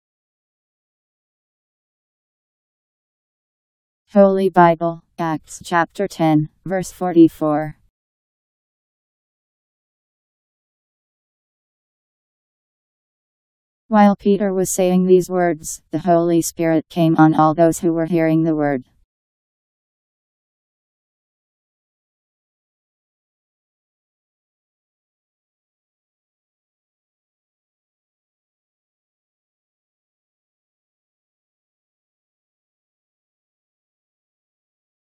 Holy Bible Acts 10:44
Video shows Holy Bible Acts Chapter 10 Verse 44: While Peter was saying these words, the Holy Spirit came on all those who were hearing the word. Powered by MaryTTS. Text taken from Bible in Basic Edition. Bible Acts 10:44 Bible Learning, what Bible verse says.